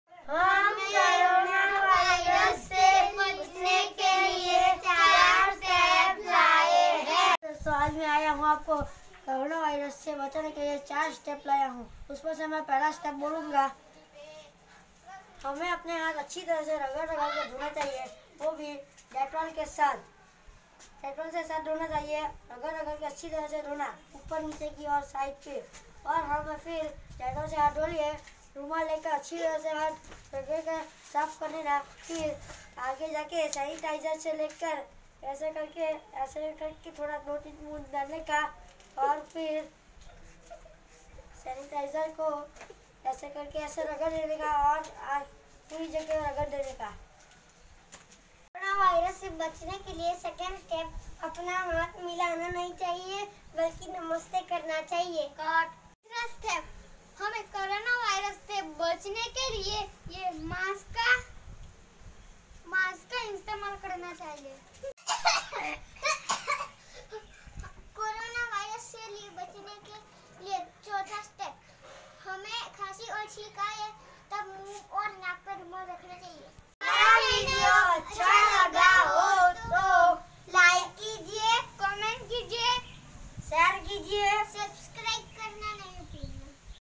Awareness Steps from Corona virus.
In this video some little children show us to How to be Aware from Corona virus.